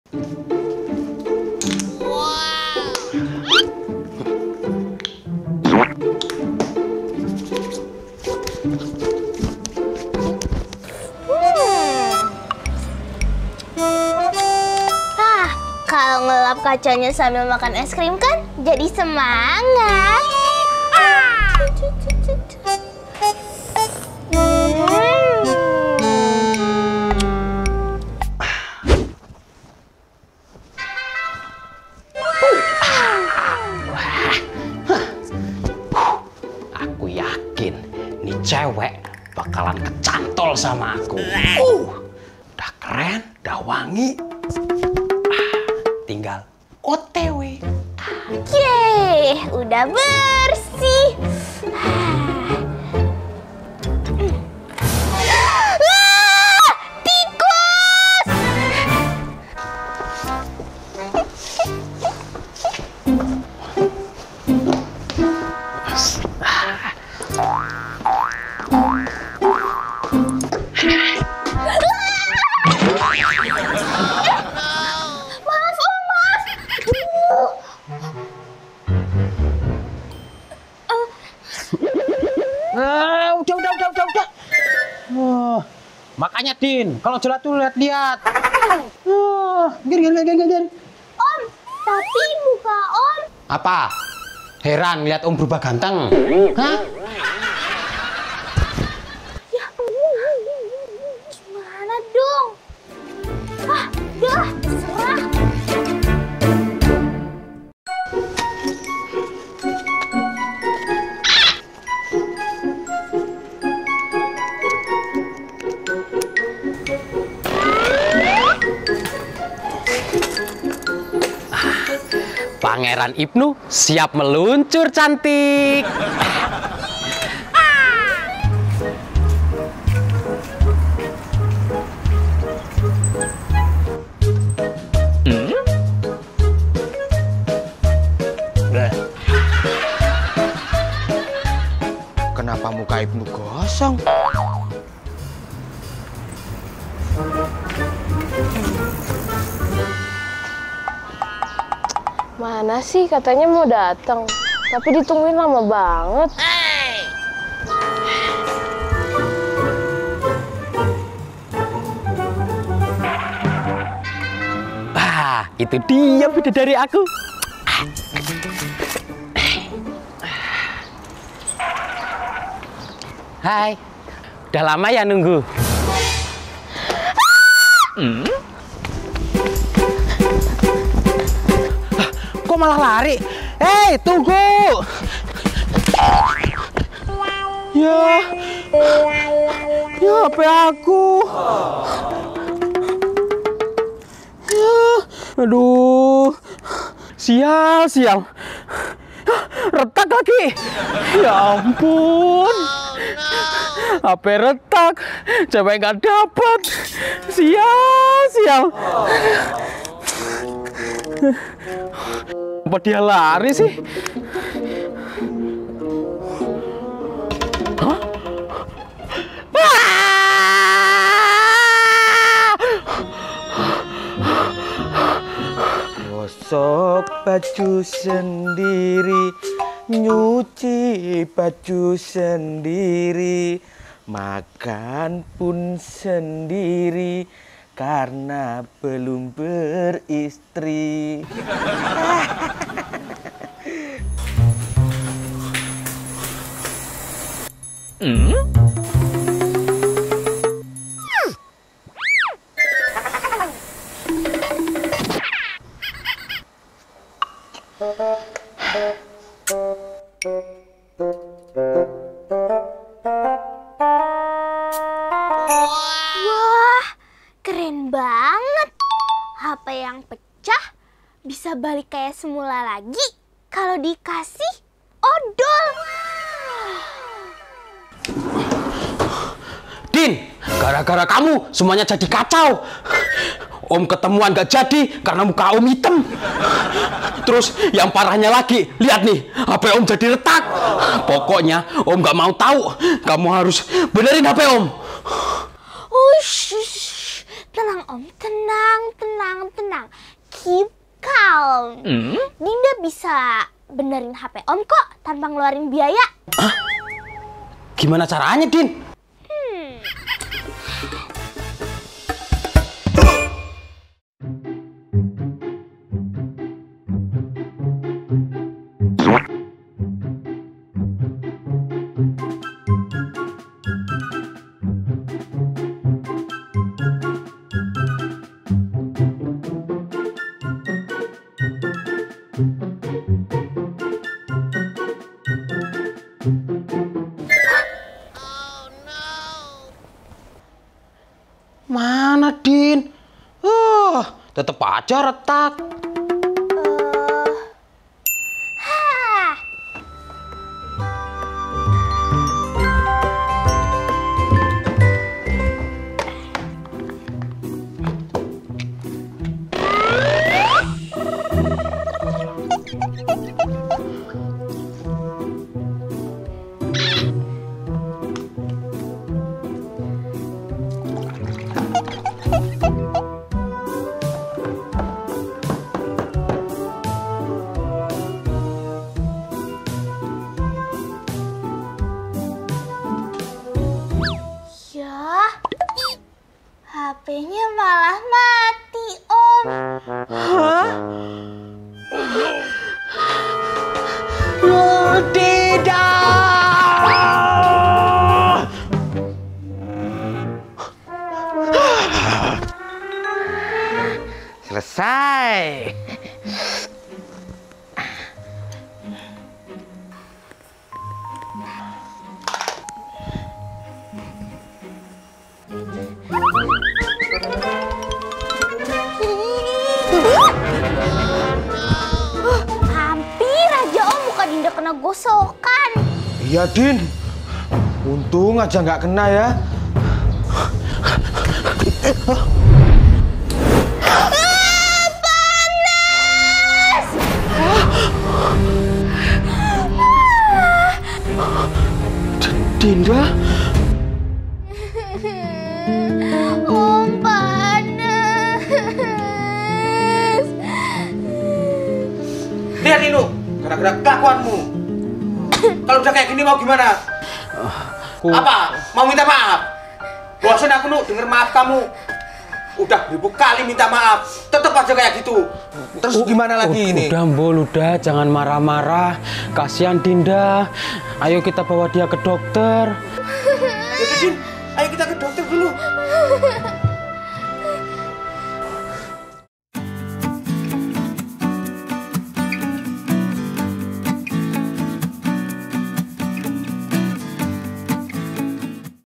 Drama || Muka Dinda Gosong Terkena Setrika
Dinda Family kali ini membuat video yang berjudul Drama || Muka Dinda Gosong Terkena Setrika.